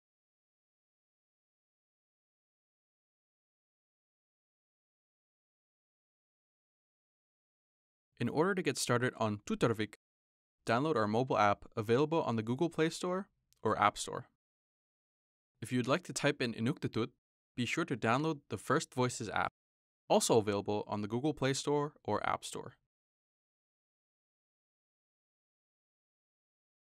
Getting Started on Tuttarvik
This video shows Inuit where to find to find Tuttarvik app on stores. If you want to use Inuktitut, you will need First Voices app.